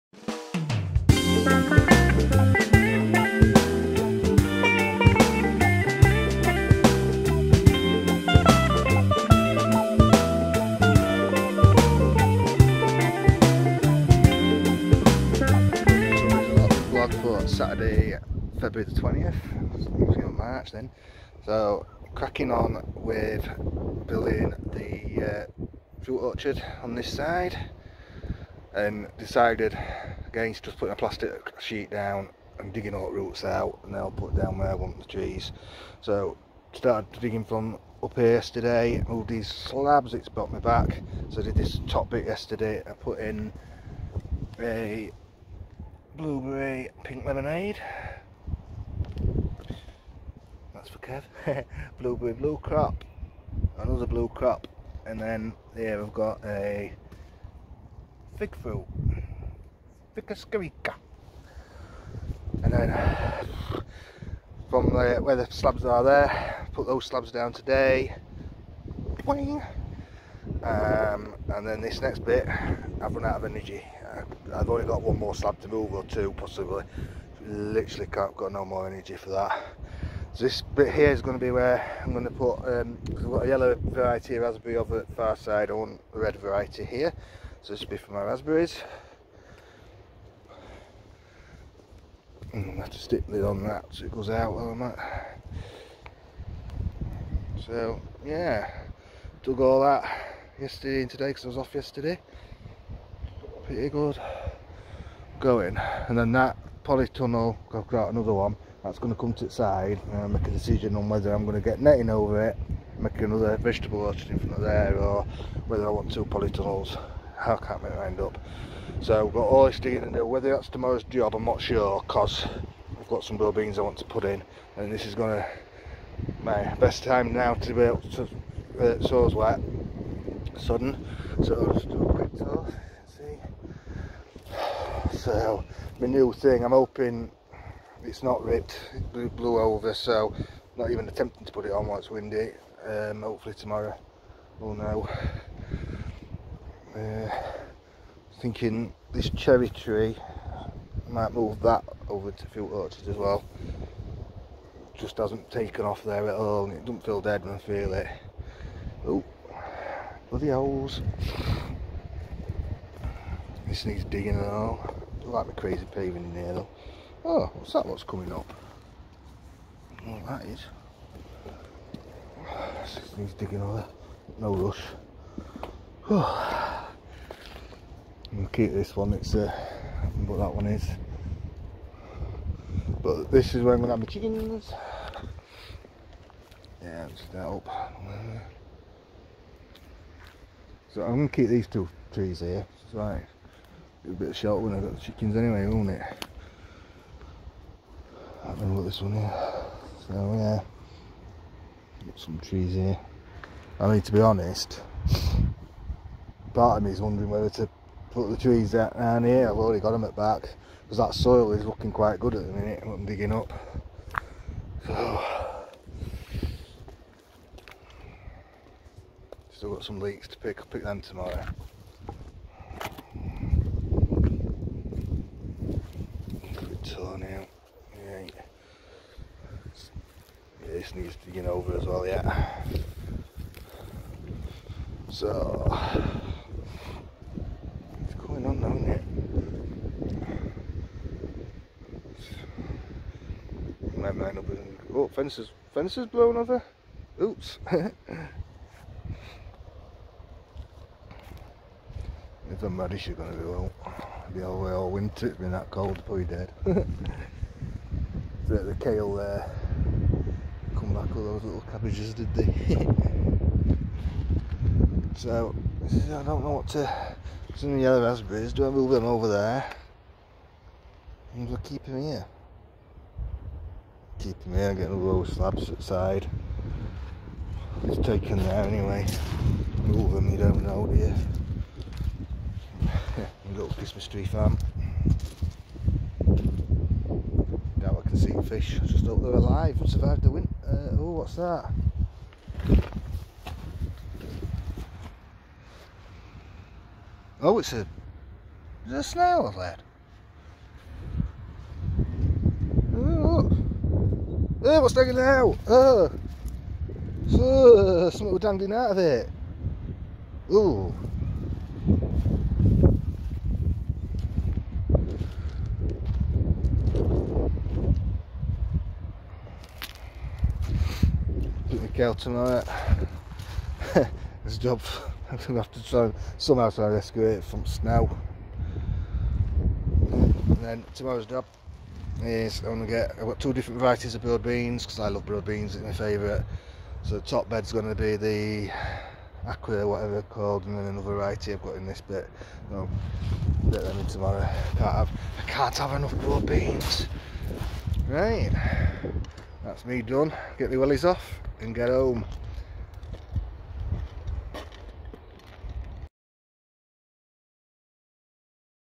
Robbys Allotment Blog 20022021
Started to build my fruit orchard side. digging n moving slabs. shattered!!!  Oh and a funny moment pond side i got a tad squeemish